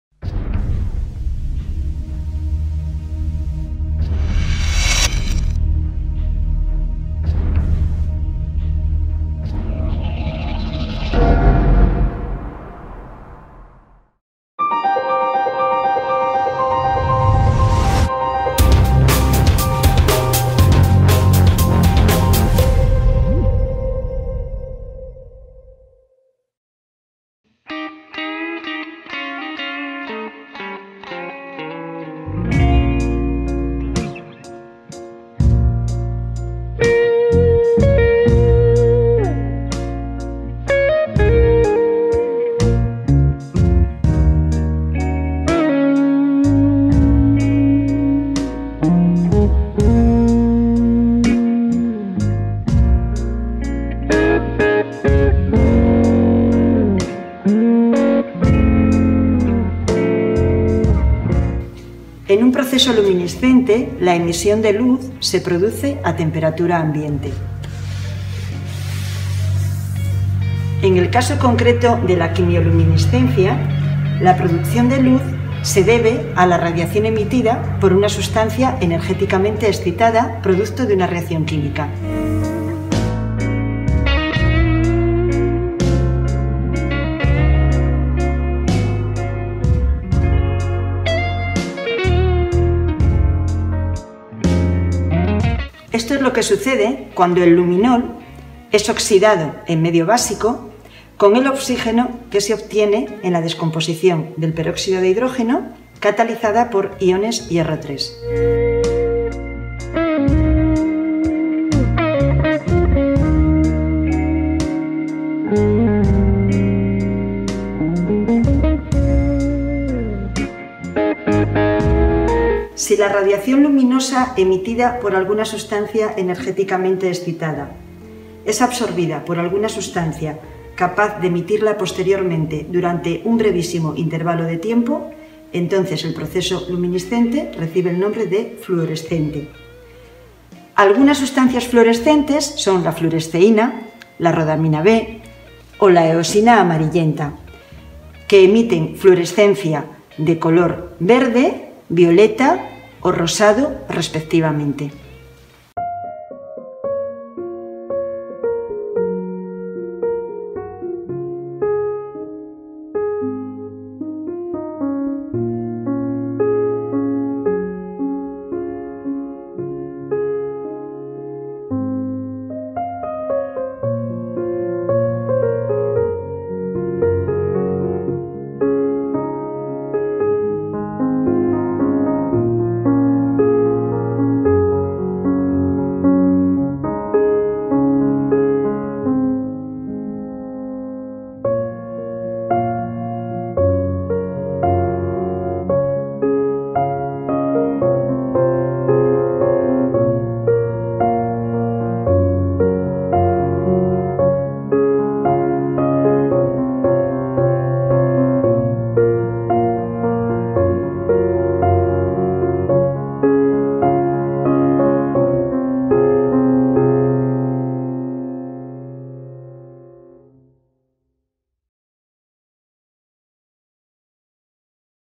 Luminiscencia
La quimioluminiscencia es un fenómeno de indudable interés estético. En reino animal siempre ha estado asociado a especies casi mágicas como las luciérnagas o ciertos tipos de medusas. Esta es una breve recreación con cuatro sugerentes tonos de color.